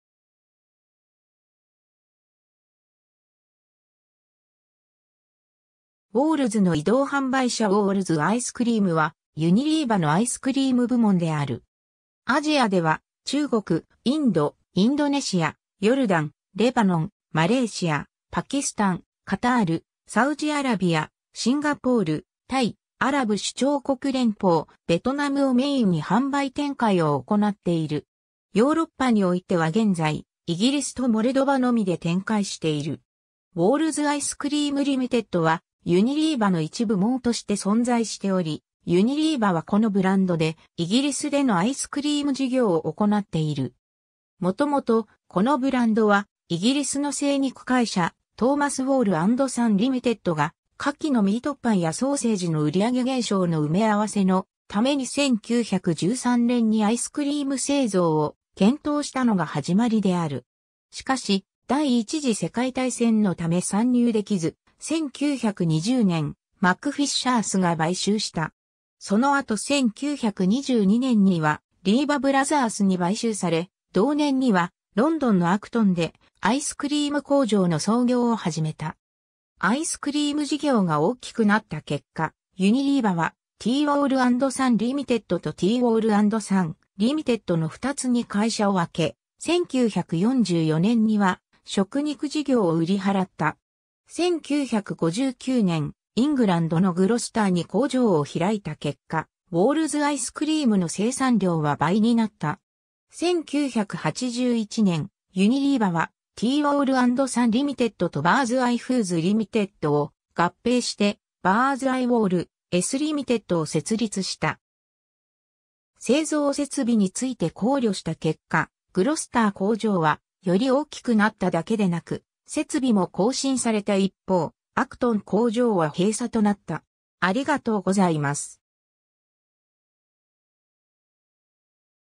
ウォールズ・アイスクリーム
ウォールズの移動販売車 ウォールズ・アイスクリーム(Wall's)は、ユニリーバのアイスクリーム部門である。
 アジアでは中国（香港を含む）、インド, インドネシア, ヨルダン, レバノン, マレーシア , パキスタン 、カタール、サウジアラビア、シンガポール、タイ, アラブ首長国連邦、ベトナムをメインに販売展開を行っている。
 ヨーロッパにおいては現在、イギリスとモルドバのみで展開している。
 ウォールズ・アイスクリーム(Wall's (Ice Cream) Ltd)は、ユニリーバの一部門として存在しており、ユニリーバはこのブランドでイギリスでのアイスクリーム事業を行っている。
 もともと、このブランドは、イギリスの精肉会社 Thomas Wall and Son Ltdが 夏期の ミートパイやソーセージの売り上げ減少の埋め合わせのために1913年にアイスクリーム製造を検討したのが始まりである。
しかし第1次世界大戦のため参入できず、1920年、MacFisheriesが買収した。
そのあと1922年にはリーバ・ブラザースに買収され、同年にはロンドンのアクトンでアイスクリーム工場の操業を始めた。
 アイスクリーム事業が大きくなった結果、ユニリーバは、 T Wall and Son (アイスクリーム部門) LtdとT Wall and Son (Meats) Ltd(食肉部門)の2つに会社を分け、1944年には食肉事業を売り払った。
 1959年、イングランドのグロスターに工場を開いた結果、ウォールズ・アイスクリームの生産量は倍になった。
 1981年、ユニリーバは T Wall and Son (アイスクリーム部門) Ltd とBirds Eye Foods Ltdを合併してBirds Eye Wall's Ltdを設立した。
 製造設備について考慮した結果、 グロスター工場はより大きくなっただけでなく設備も更新された一方、アクトン工場は閉鎖となった("Project Phoenix" 1983)。